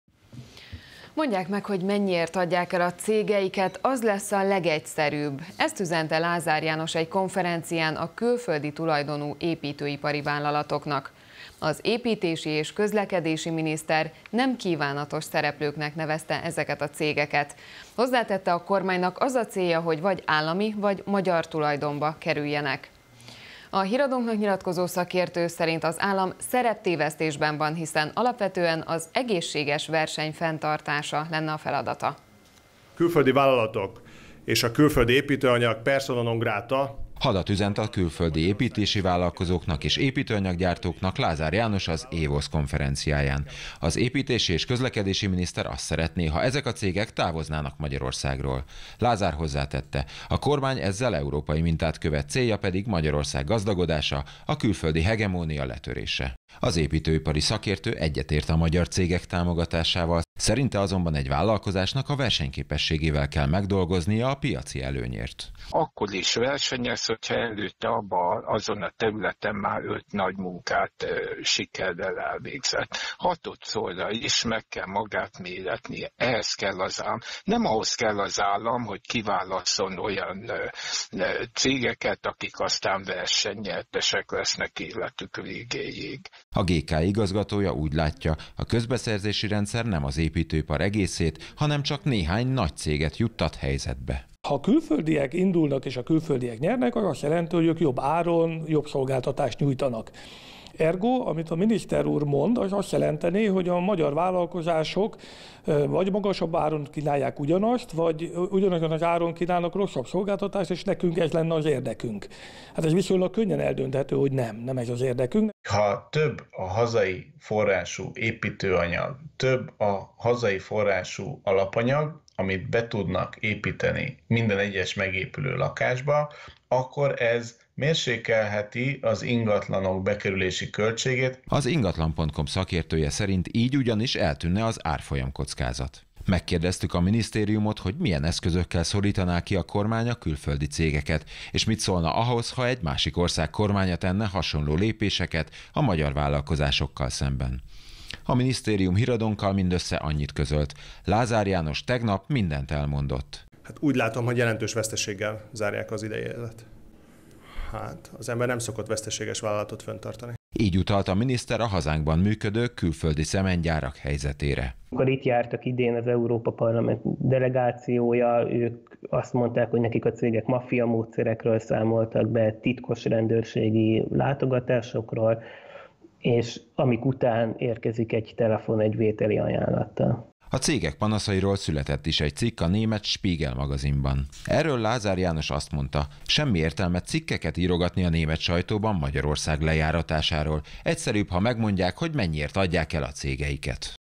Lázár János hadat üzent a külföldi építőipari cégeknek: „semmi keresnivalójuk nincs Magyarországon”
„Mondják meg, hogy mennyiért adják el a cégeiket, az lesz a legegyszerűbb” - ezt üzente Lázár János egy konferencián a külföldi tulajdonú építőipari vállalatoknak. Az építési és közlekedési miniszter nemkívánatos szereplőknek nevezte ezeket a cégeket. Hozzátette: a kormánynak az a célja, hogy vagy állami, vagy magyar tulajdonba kerüljenek.